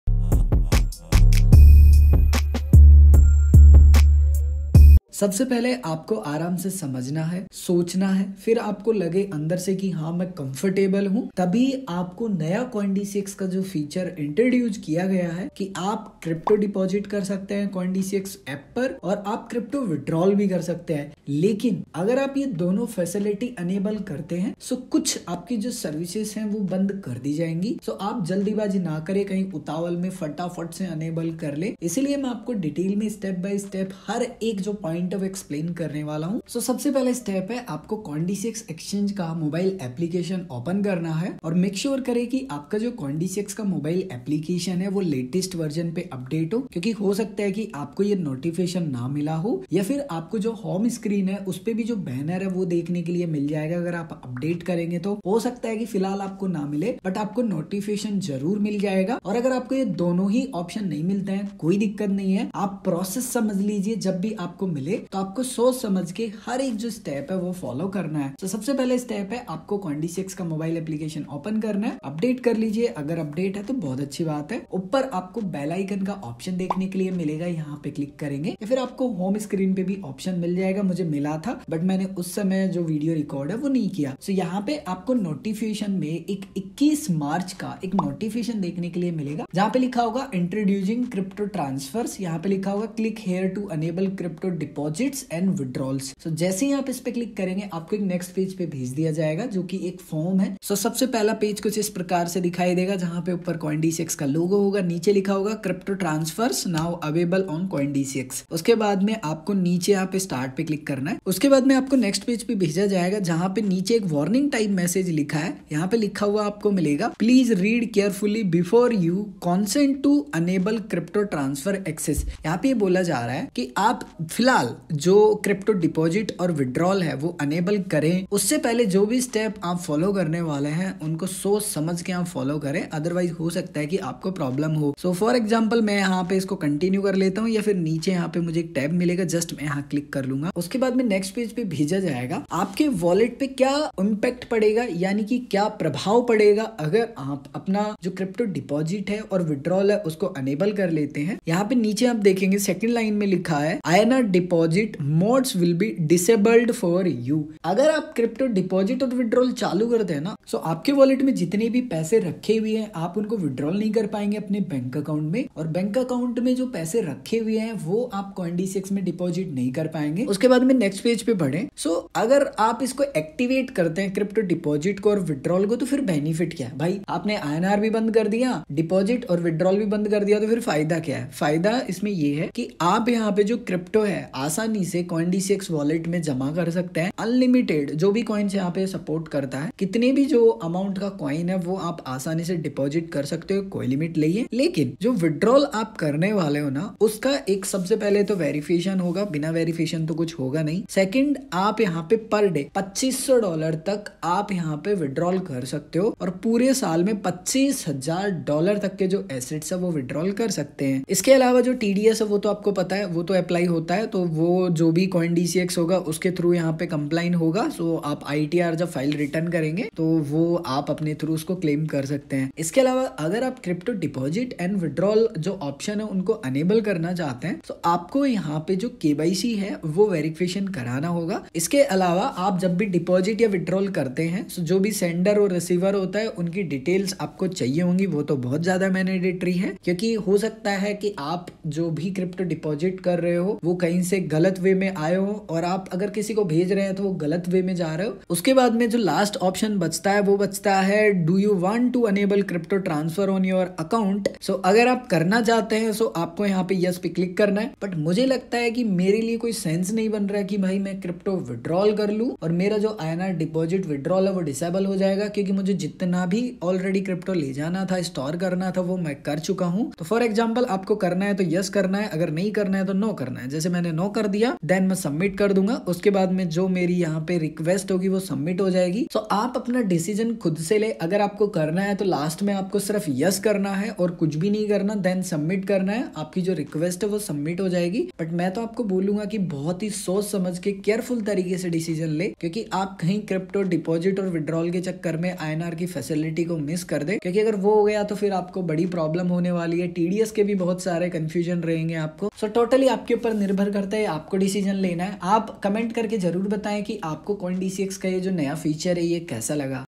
CoinDCX Exchange Crypto Deposit & Withdraw Live | Early Access form Live
CoinDCX Exchange New Product Okto launch | Okto Crypto Self Custody Wallet

3. Okto - Self Custody Wallet
Okto aims to:
- Uncomplicate investing in DeFi
- Uncomplicate multichain experience
- Uncomplicate wallet security

🙋🏻‍♂️Open Your ZERO Brokerage Trading & Demat Account

⚠️Also, remember we never ask for money don't have paid courses or paid groups. We do everything for free, so if someone asks for money - That's a scammer